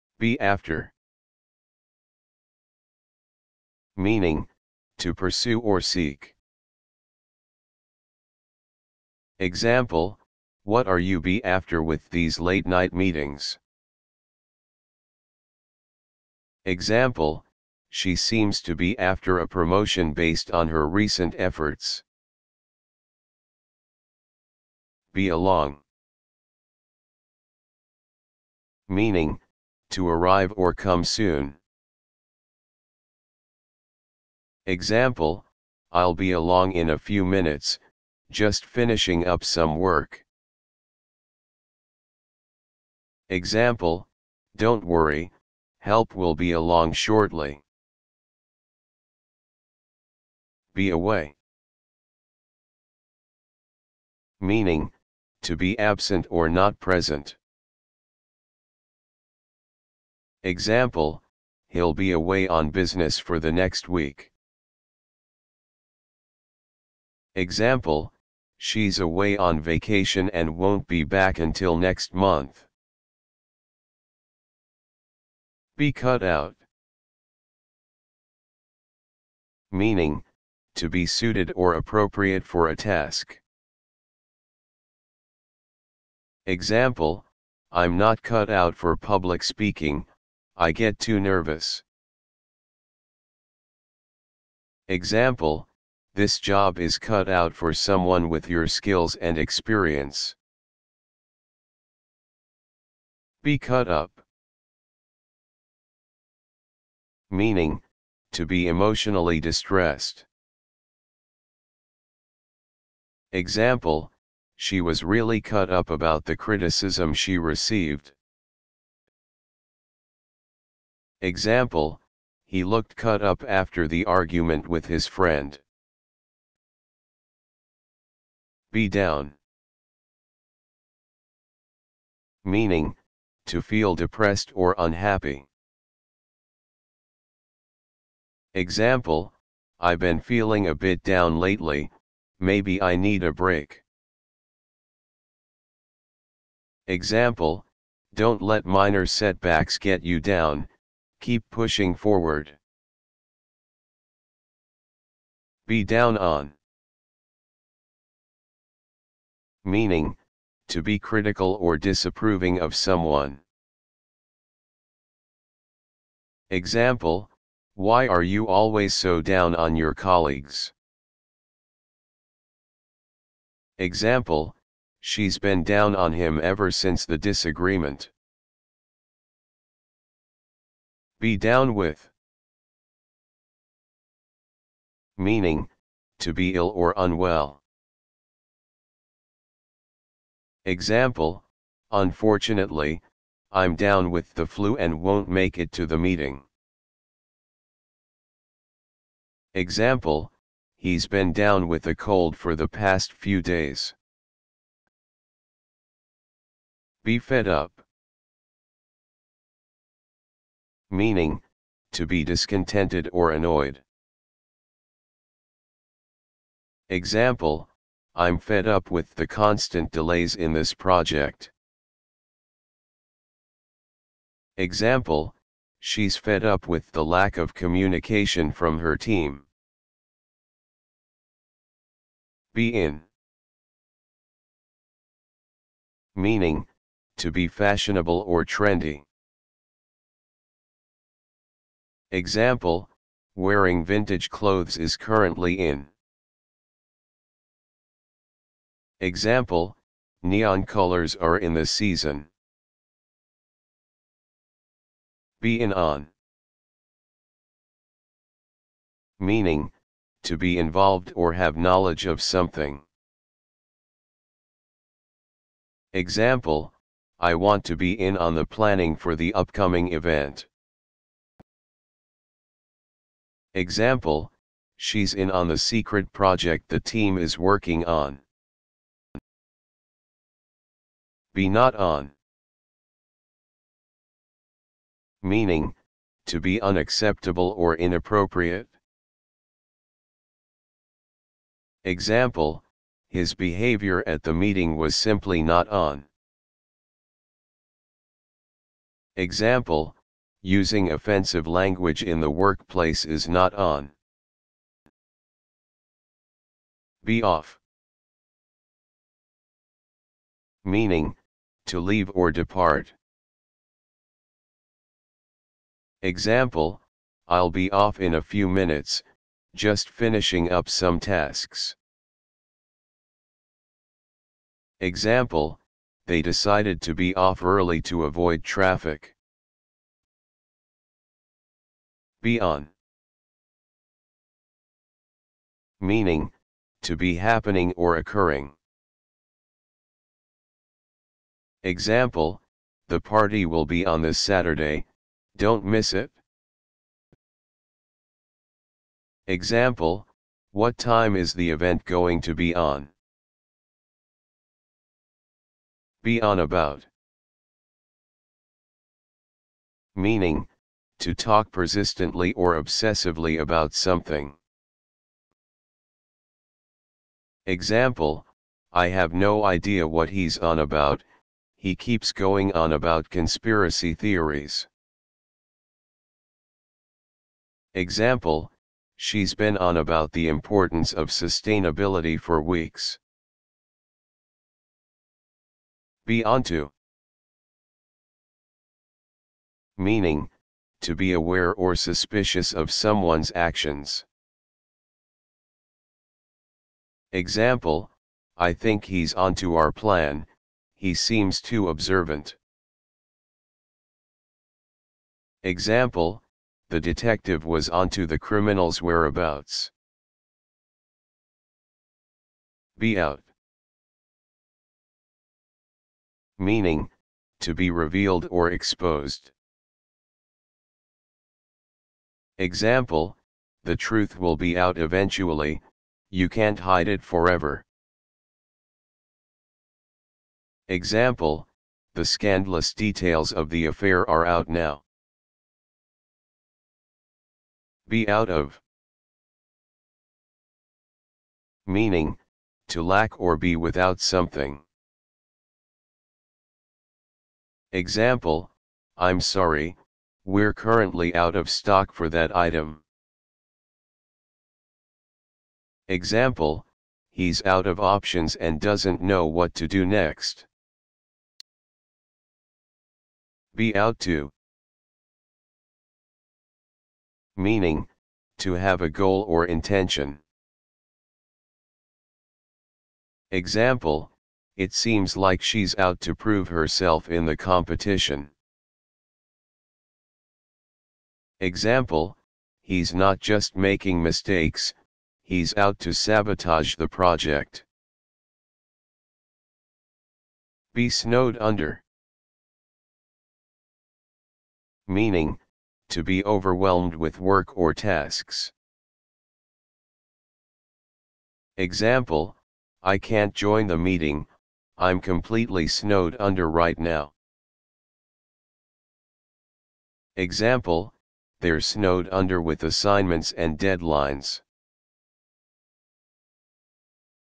20 English Phrasal Verbs | Commonly used Phrasal Verbs for everyday life | Learn English List 4
20 English Phrasal Verbs | Commonly used Phrasal Verbs for everyday life | English Vocabulary

List of commonly used english phrasal verbs to enhance your english Vocabulary

This video is part 4 of commonly used phrasal verbs in everyday life.

Phrasal Verbs for daily use:
Be after
Be along
Be away
Be cut out
Be cut up
Be down
Be down on
Be down with
Be fed up
Be in
Be in on
Be not on
Be off
Be on
Be on about
Be onto
Be out
Be out of
Be out to
Be snowed under